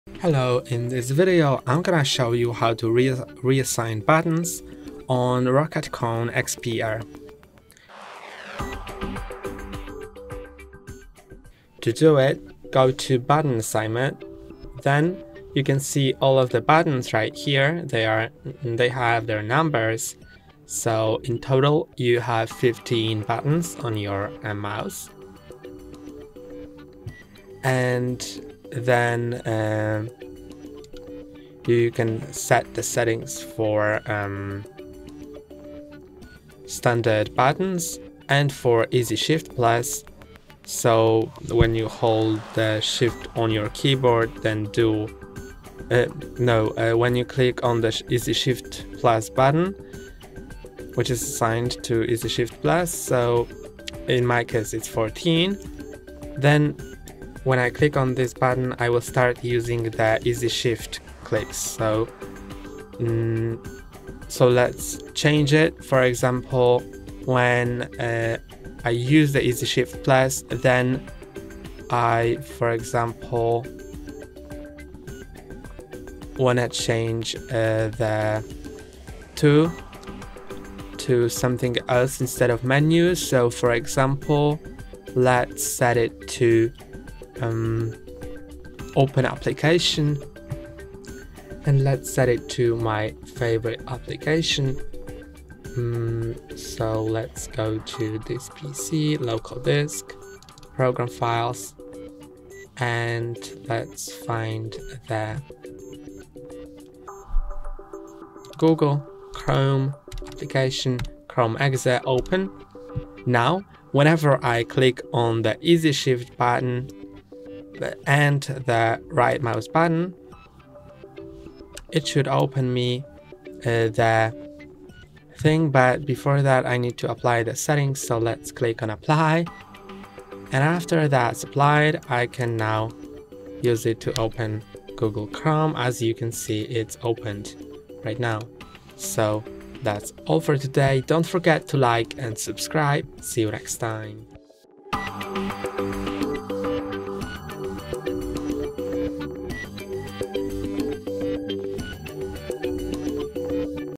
ROCCAT KONE XP AIR - How to Reassign Buttons - Customization Guide
In this tutorial, we’ll show you how to reassign buttons on your ROCCAT KONE XP AIR gaming mouse. Whether you want to remap buttons for gaming shortcuts, productivity, or personal preferences, customizing your mouse buttons can give you the ultimate control over your tasks.

Step-by-step guide to reassigning buttons on the ROCCAT KONE XP AIR
How to use the ROCCAT Swarm software to remap buttons for different functions
Tips for setting up macros or custom commands for your most-used actions
How to save and manage multiple button profiles
Troubleshooting tips if reassigned buttons are not working correctly